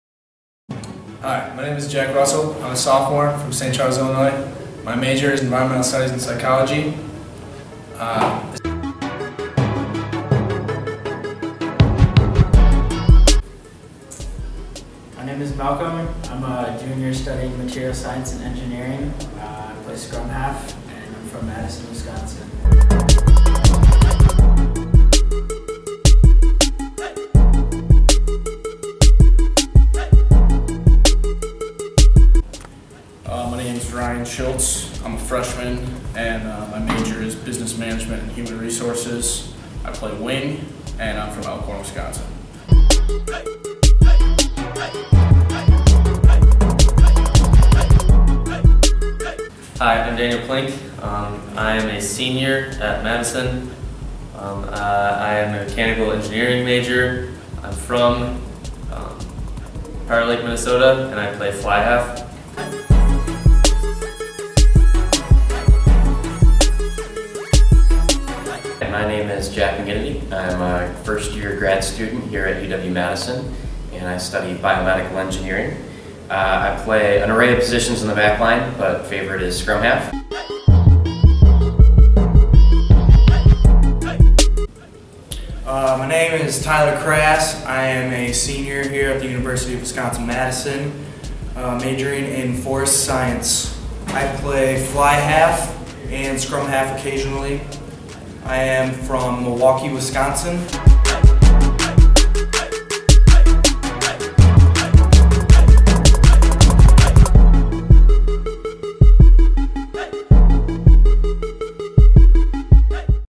Meet the team- Backs
A look at some of the faces of our 7's squad